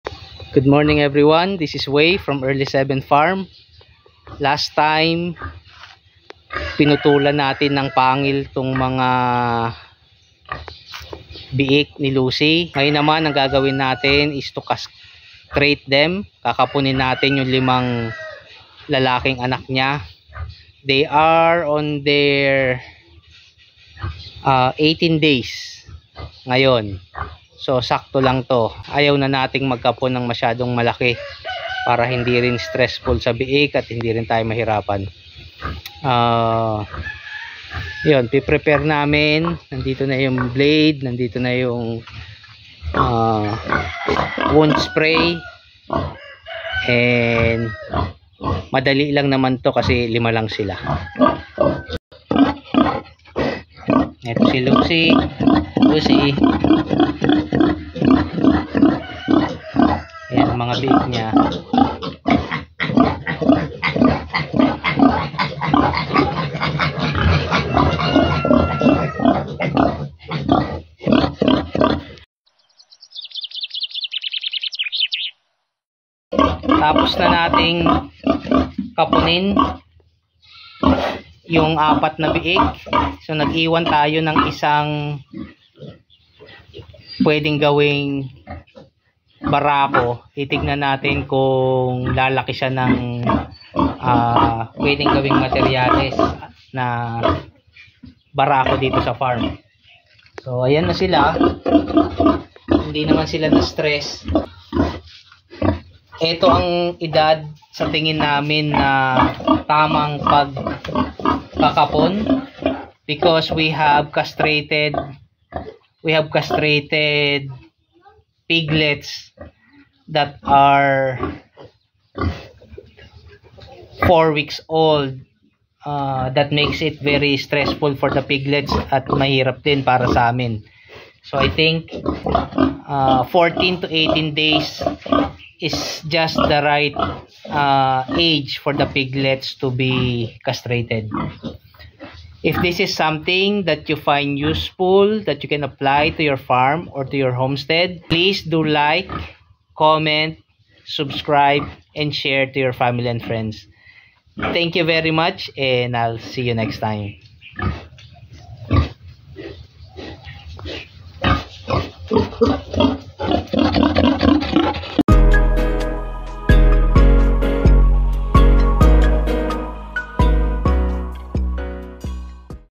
At what age we castrate our piglets?
We have castrated piglets at diiferent ages. And the best number of days as per our experience is at 14 to 18 days old. it is less stressful for the piglets and easier for us to handle them.